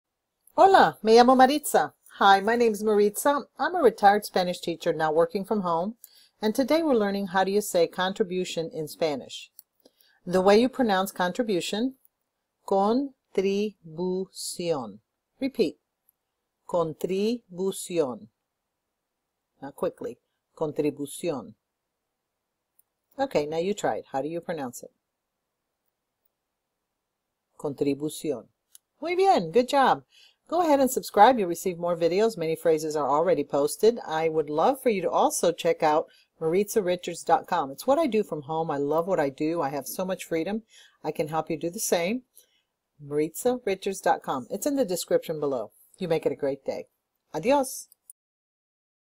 How Do You Say Contribution In Spanish
Learn How Do You Say Contribution In Spanish

Be able to tell someone Contribution In Spanish
Contribucion

to learn about social media and Smashfund and Crowdfunding